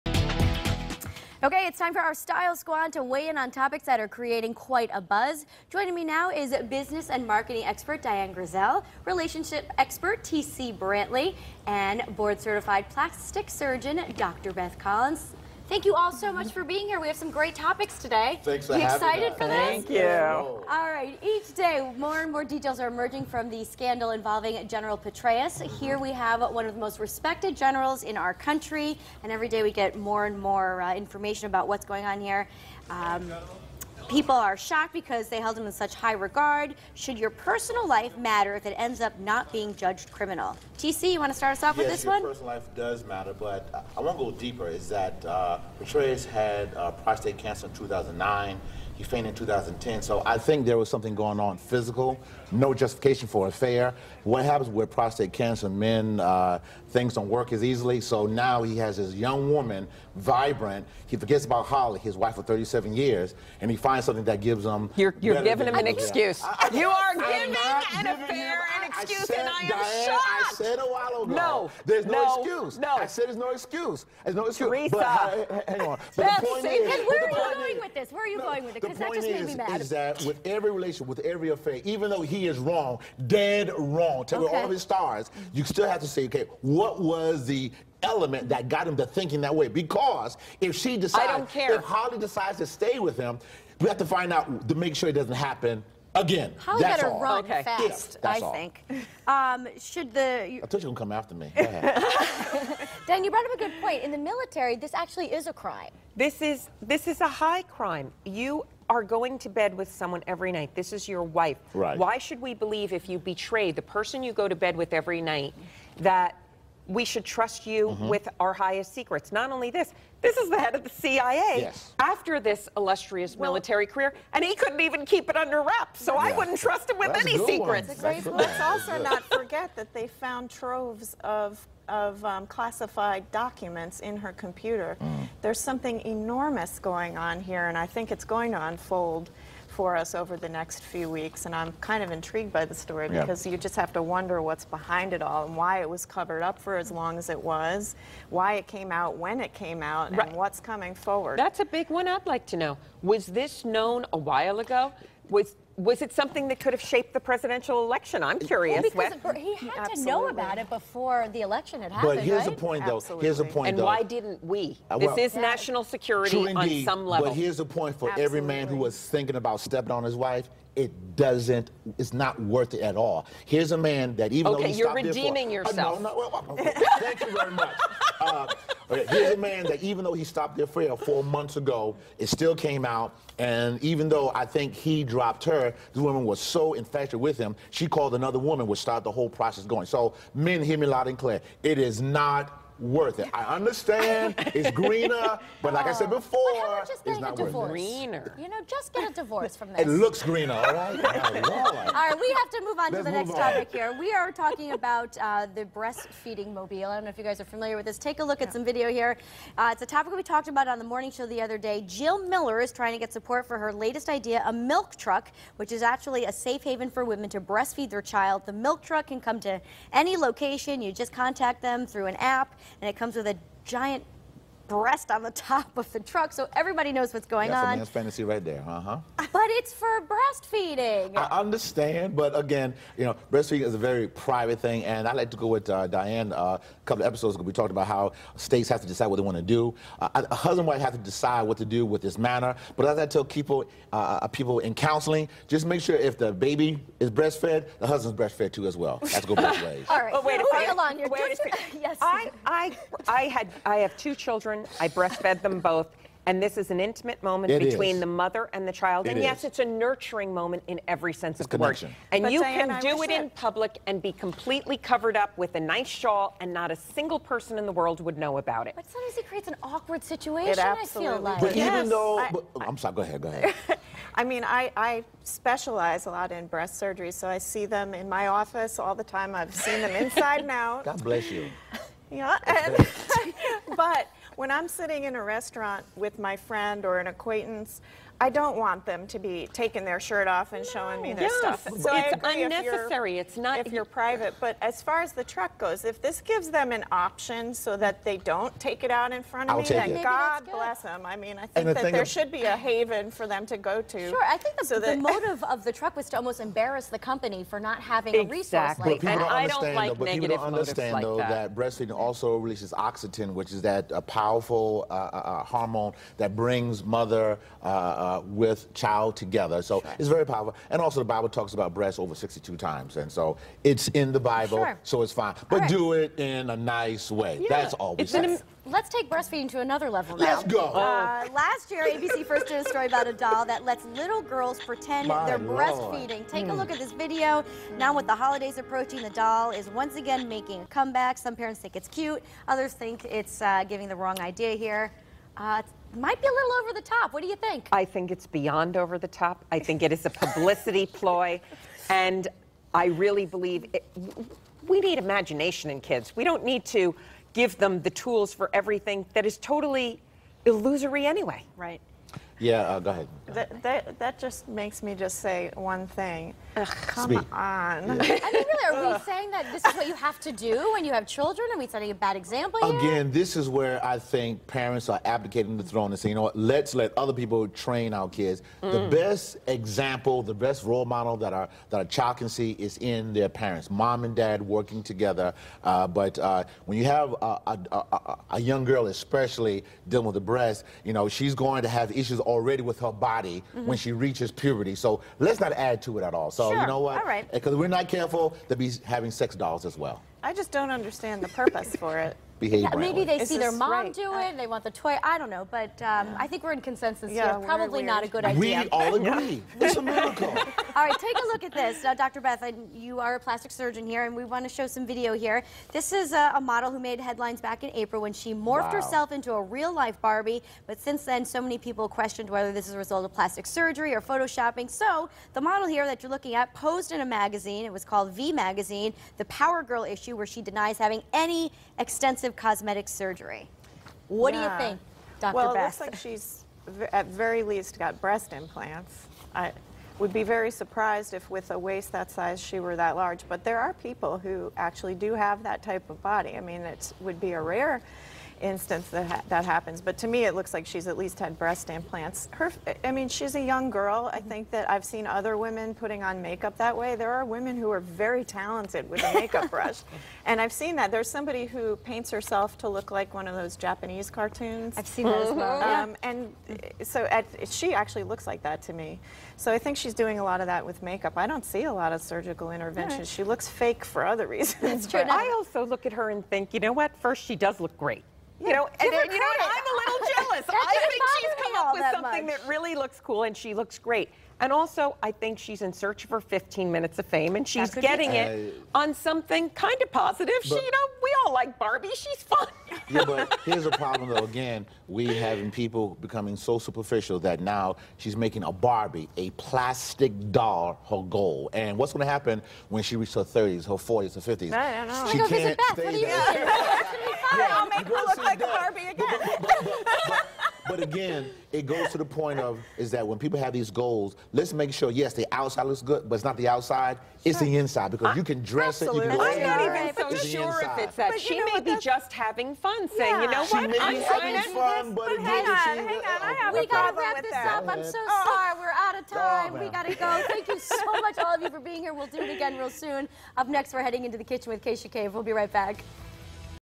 November Style Squad
Business & Marketing Expert, Dian Griesel, Relationship Expert, T.C. Brantley, and Board Certified Plastic Surgeon, Beth Collins, join Teresa in the studio to discuss topics such as the General Petraeus Scandal, the Breast Feeding Milk-Mobile, the Breast Feeding Doll for Girls, and the Model who has morphed herself into a real-life Barbie.